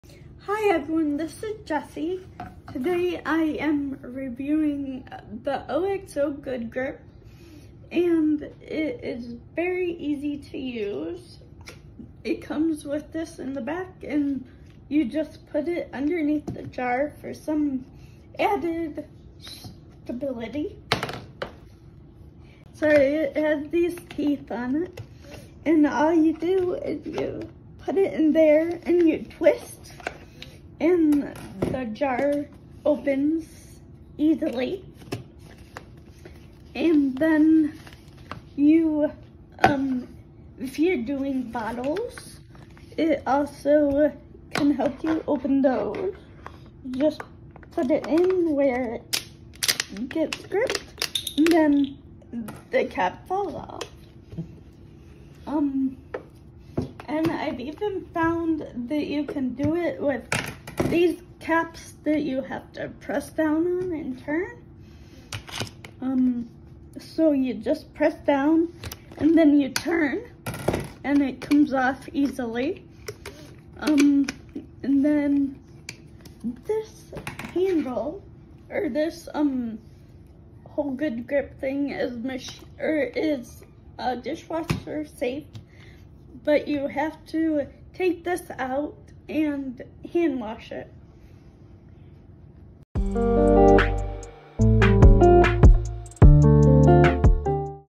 Using the OXO Jar Opener with Base Pad
Jessica reviews everyday products for accessible living at Day Undefined. In this video, she demonstrates using the OXO Good Grips Jar Opener with Base Pad. She finds this one easier to use than another jar opener she reviewed; she can even use it to open childproof containers.

Everything that we share at Day Undefined has a real person and experience behind it.

Video description: Jessica shows you how she uses the OXO Good Grips Jar opener and the base pad to open a jar of peanut butter, a water bottle, and even a childproof vitamin bottle.